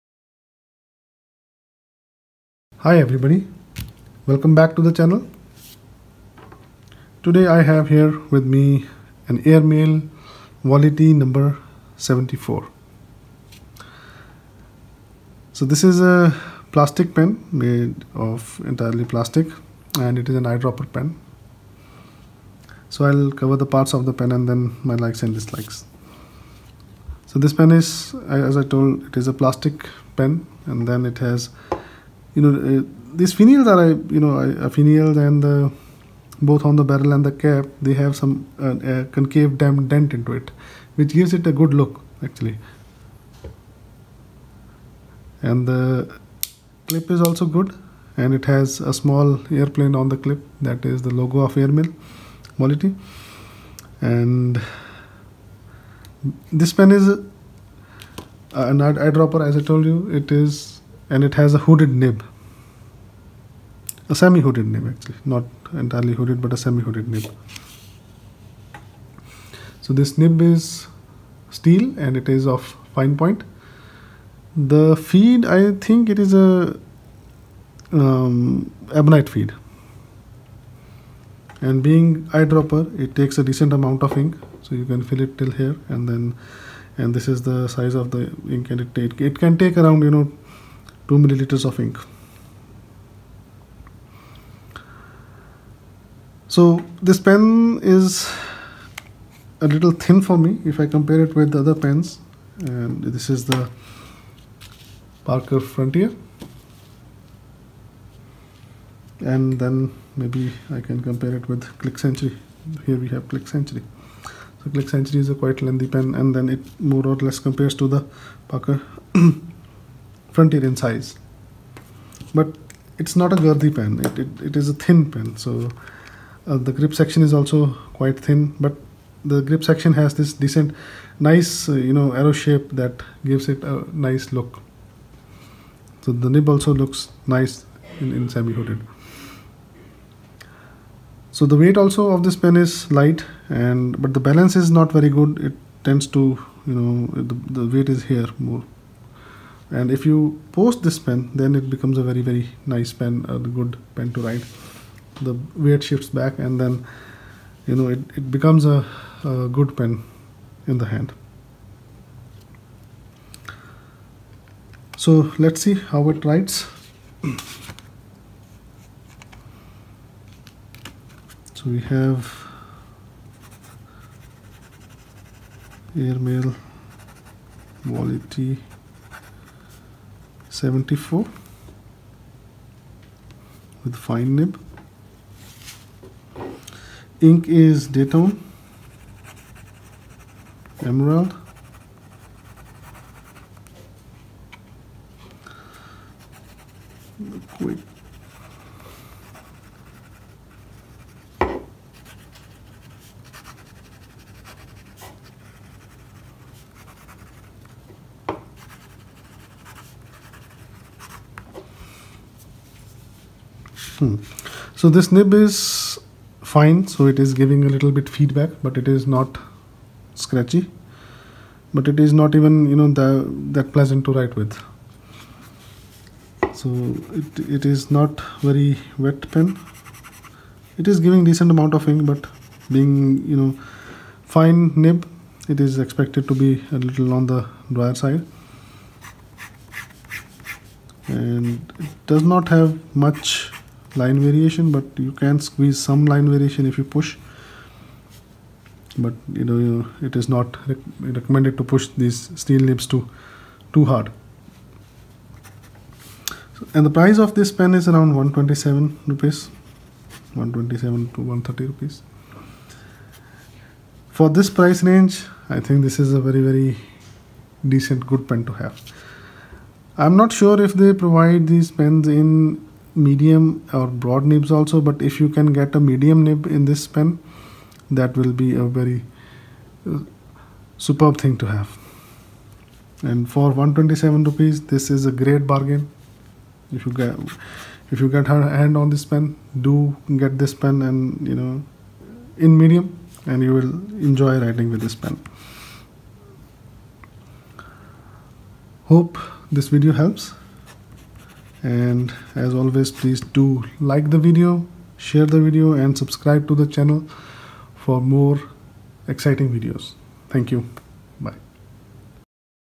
Airmail Wality 74 Fountain pen review
Here I bring you another interesting eyedropper fountain pen from Airmail Wality.